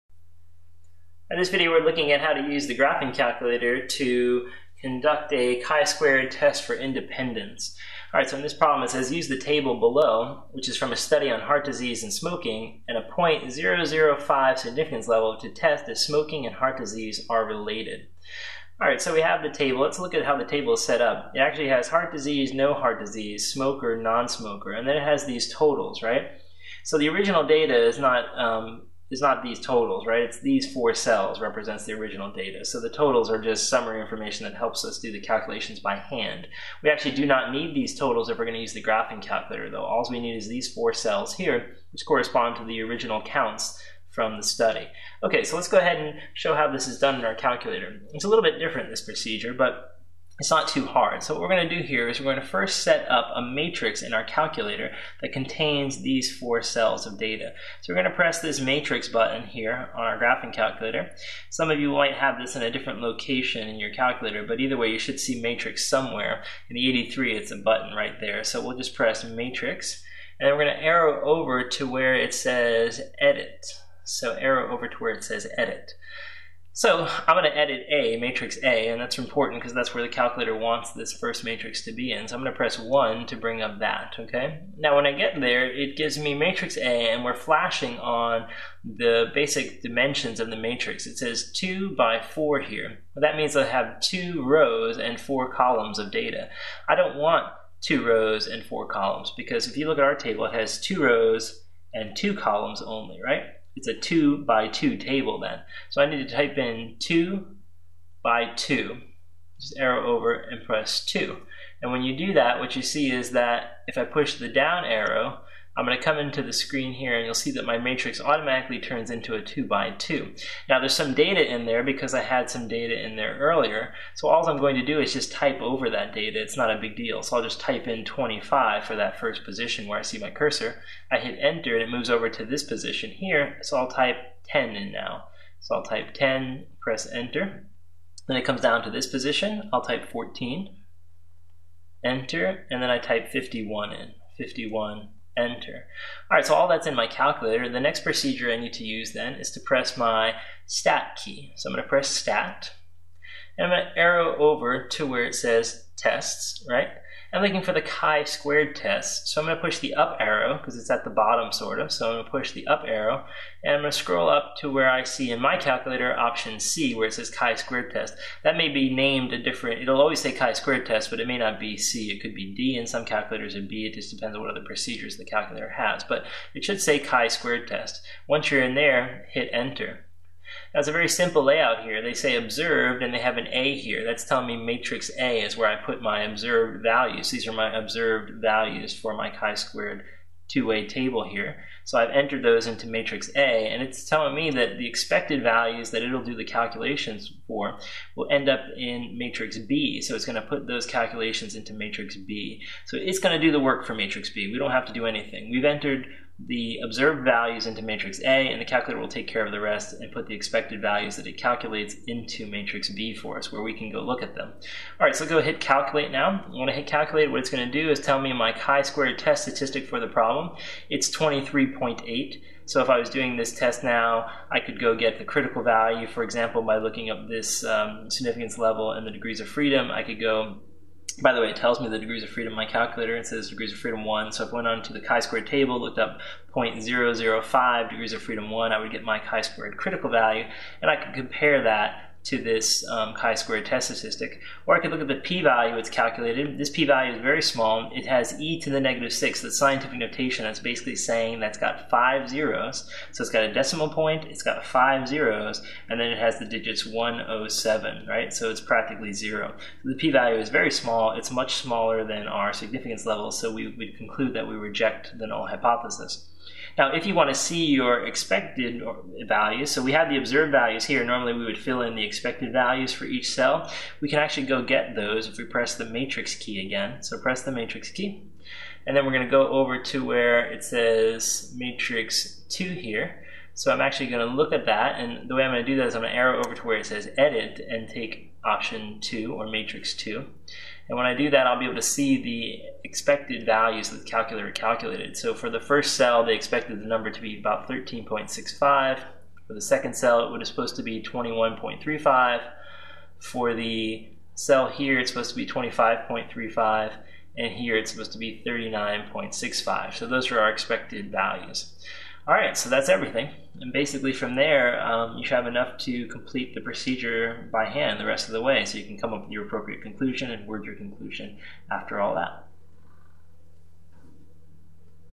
Graphing Calculator Used to Perform Chi-Square Test of Independence
In this video, we demonstrate how to conduct a chi-square test of independence using the TI-83 graphing calculator.